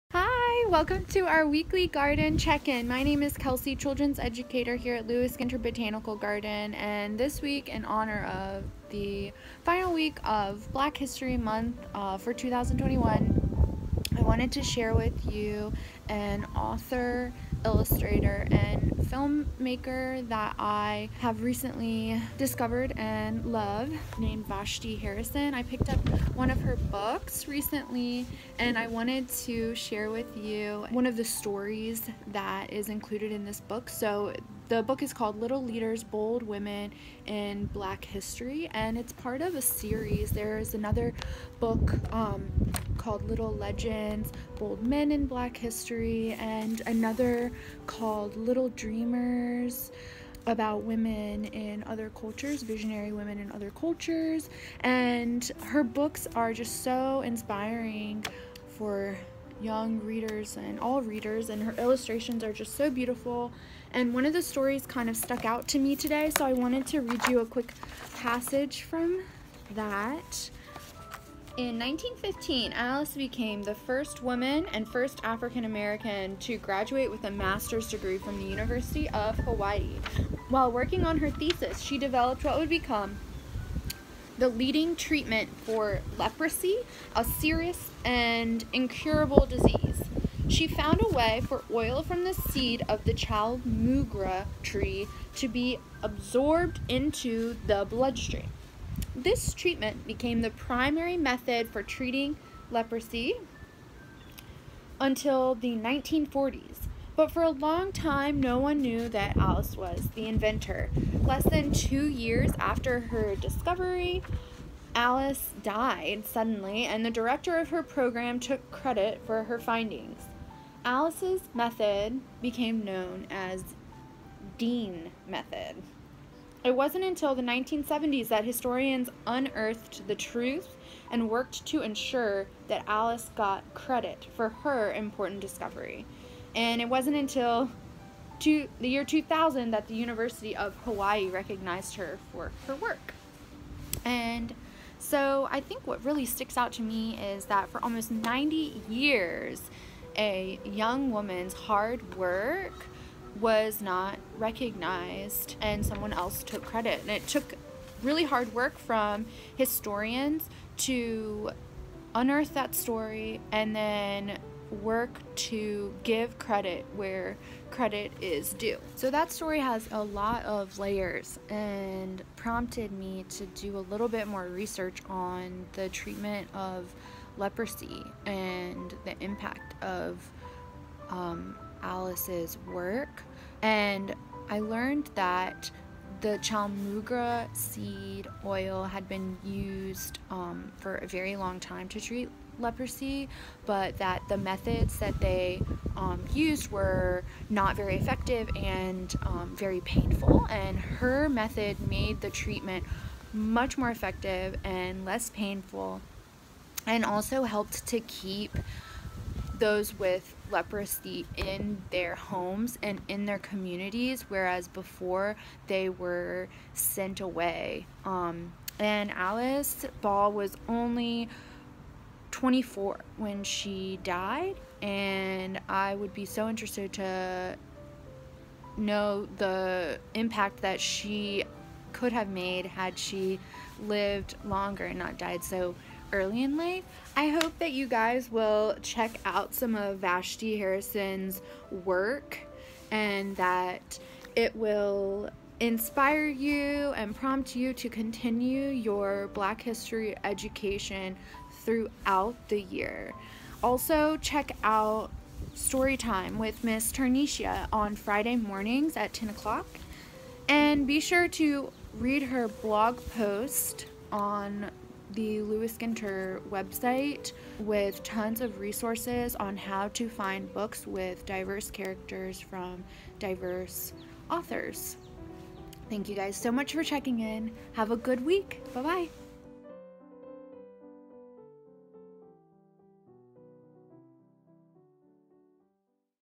The Story of Alice Ball
For the final Garden Check-In during Black History Month, Kelsey shares author, illustrator and filmmaker, Vashti Harrison's book, "Little Leaders: Bold Women in Black History." We learn about Alice Ball's contributions as she was the first woman and first African American to graduate from the University of Hawaii with a master's degree. She developed the leading treatment for leprosy using seed from the chaulmoogra tree (Hydnocarpus wightianus) but was not credited until almost 90 years later. We hope Alice's story inspires you to read the rest of the stories in Vashti Harrison's books and to continue your black history education throughout the year.